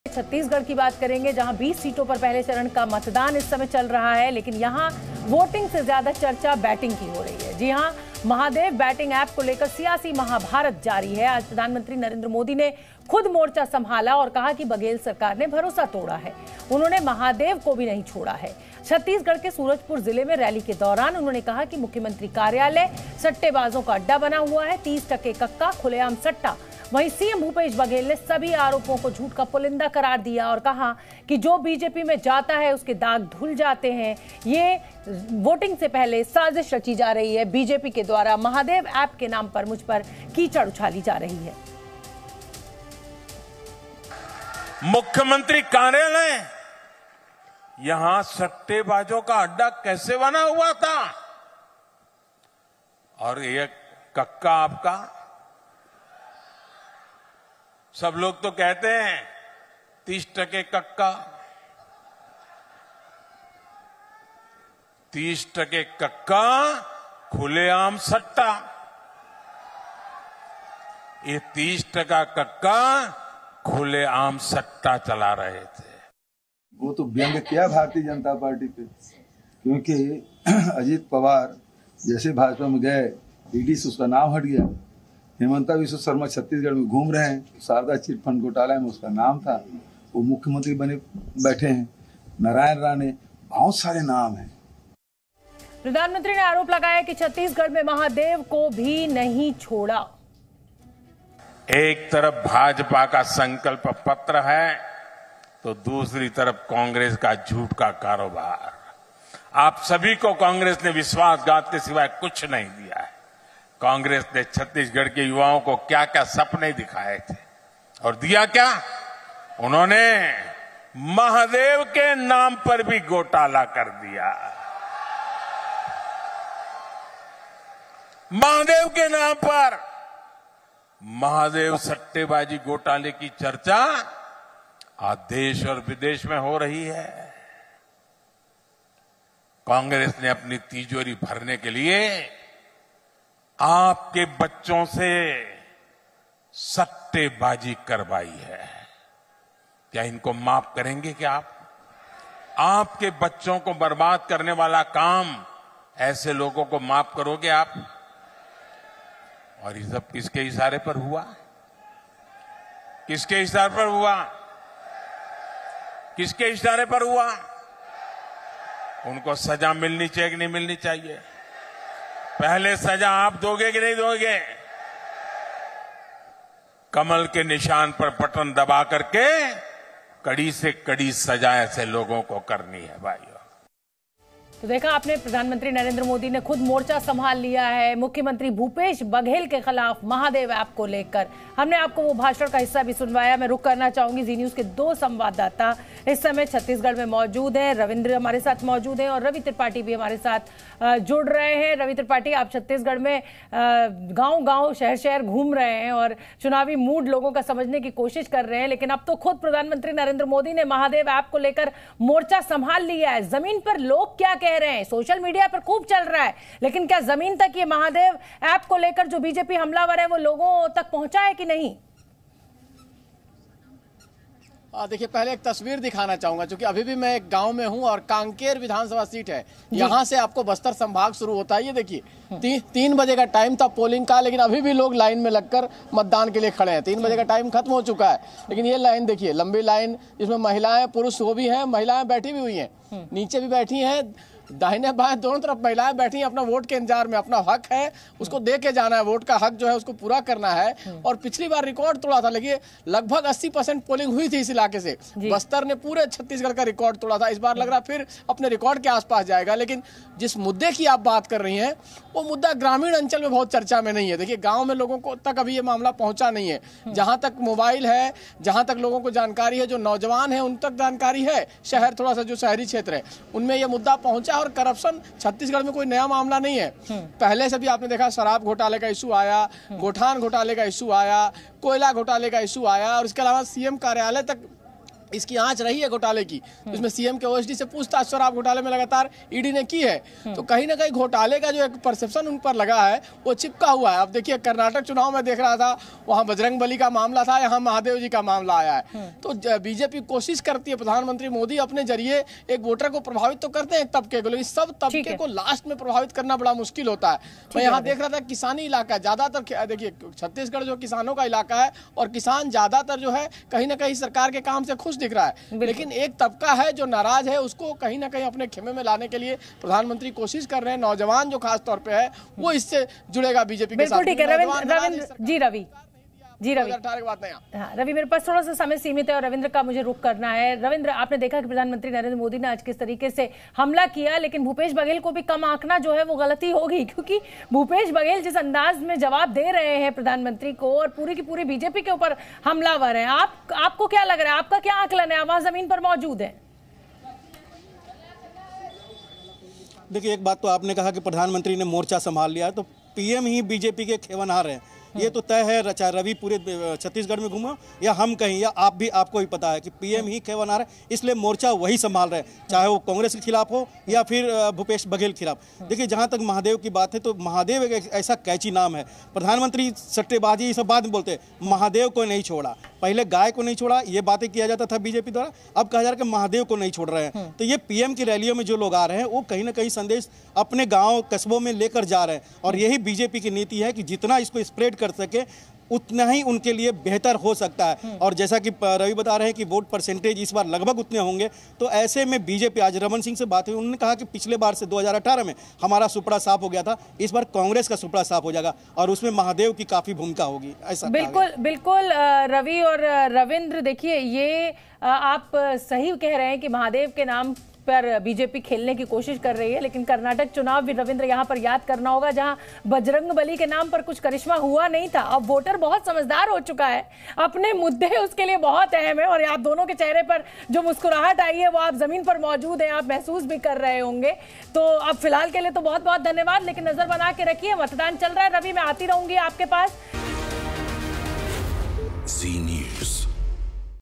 PM Modi on Mahadev App Case: '30 टके कक्का, खुलेआम सट्टा'- महादेव ऐप मोदी का हमला | Bhupesh Baghel
PM Modi on Mahadev App Case: आज प्रधानमंत्री नरेंद्र मोदी ने छत्तीसगढ़ के सूरजपुर में एक रैली को संबोधित किया। इस दौरान उन्होंने भूपेश बघेल सरकार पर गंभीर आरोप लगाया। पीएम मोदी ने महादेव ऐप को लेकर छत्तीसगढ़ के सीएम पर हमला

Today Prime Minister Narendra Modi addressed a rally in Surajpur, Chhattisgarh. During this, he made serious allegations against the Bhupesh Baghel government. PM Modi attacks Chhattisgarh CM over Mahadev app